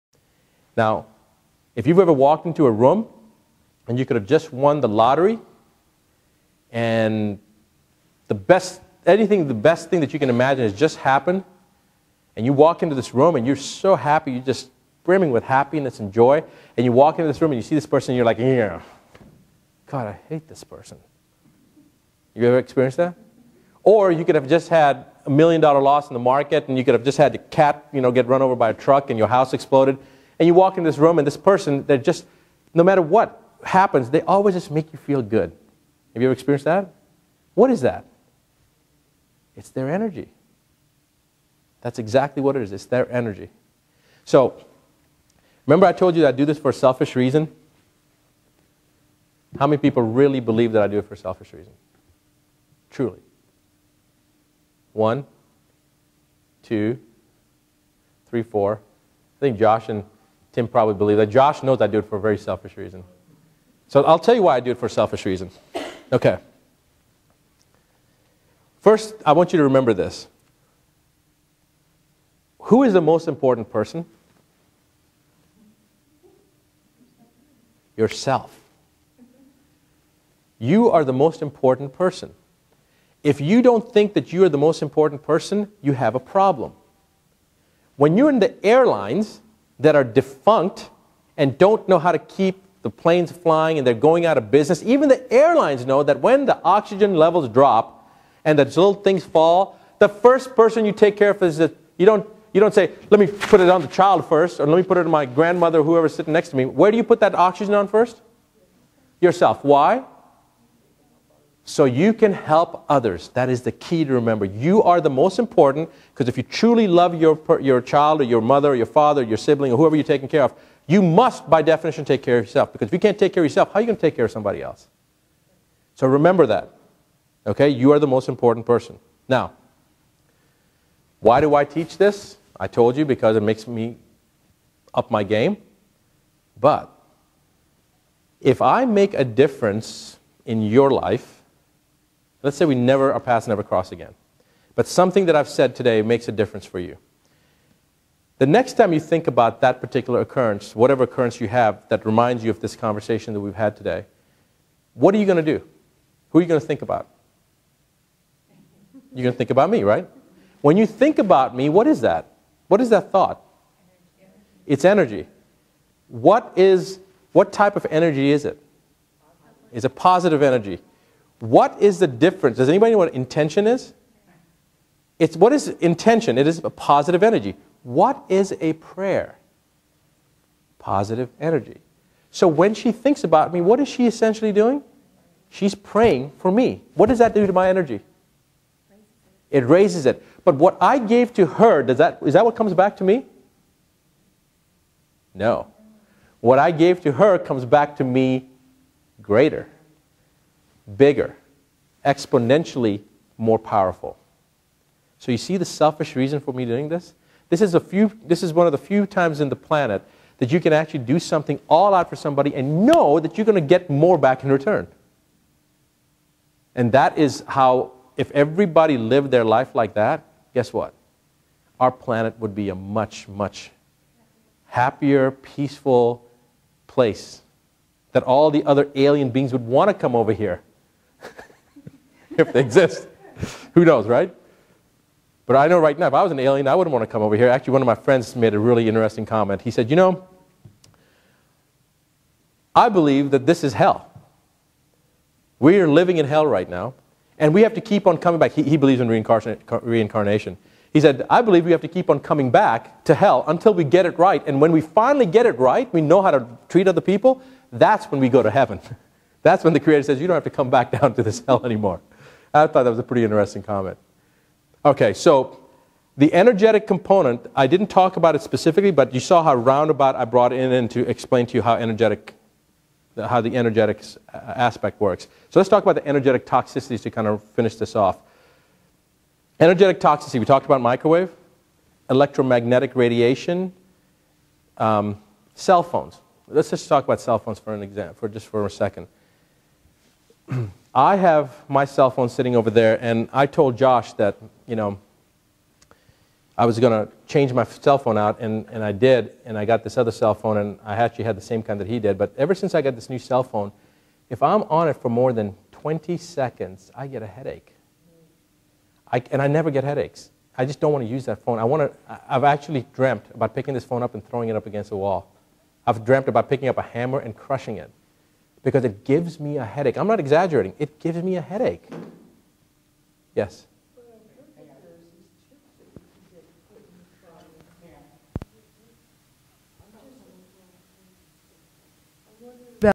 Energetics Part 13
Energetics. Side Effects of Microwave Ovens. Energy Medicine. Heavy Metal Toxicity. Healthy food is essential. Know Your Options. The Chemicals that are Killing us. Chelation and Restoring Overall Health. Healing and Energy. Dr. Rashid A. Buttar